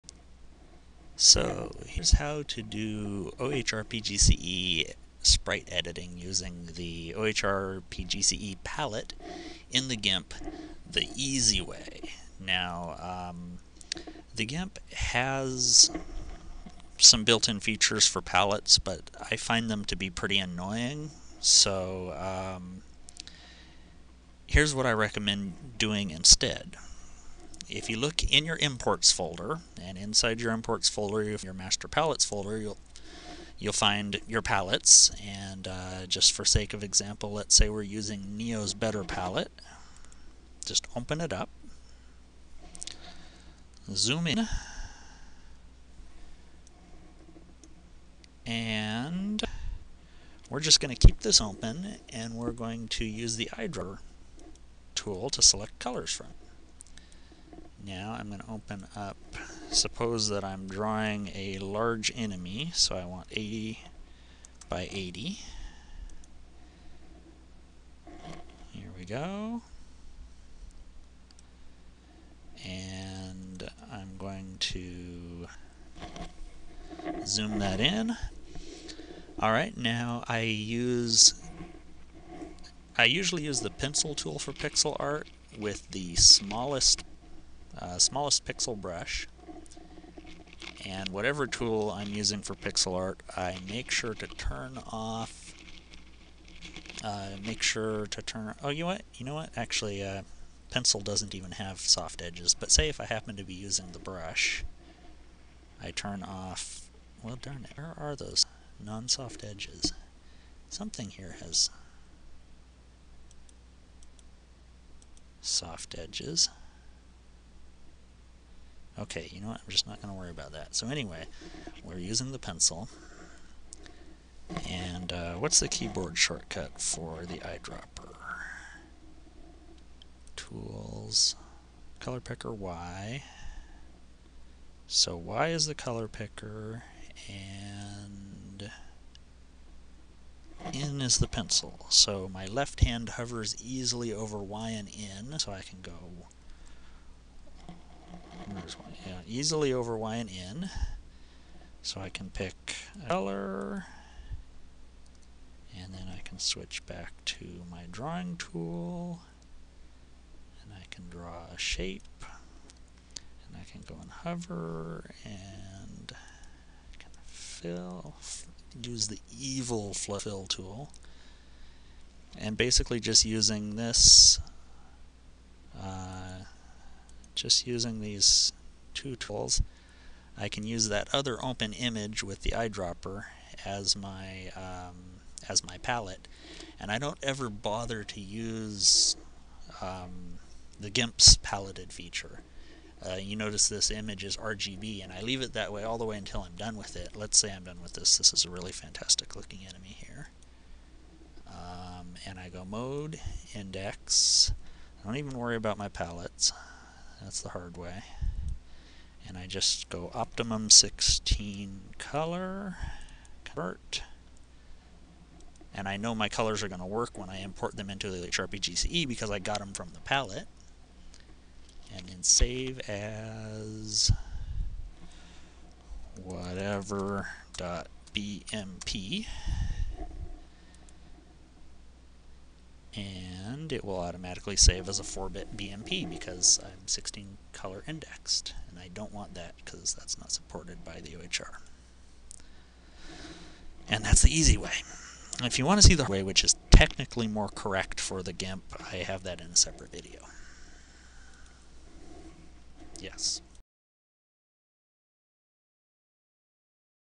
OHRRPGCE Palette in the Gimp (EASY WAY)
This video shows my preferred method of doing pixel art for the OHRRPGCE using the Gimp.

Note that it is possible to import an OHRRPGCE palette into the Gimp's palette management system, but I generally think it is more trouble than it is worth, which is why I doit this way instead. Nevertheless, if you want to see how to import the palette,